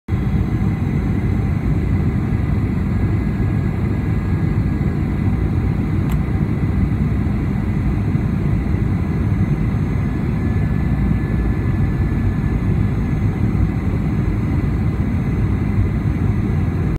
Landing on Adak Island, Alaska
Alaska Airlines approaches Adak Island through the fog and low ceiling in May 2016.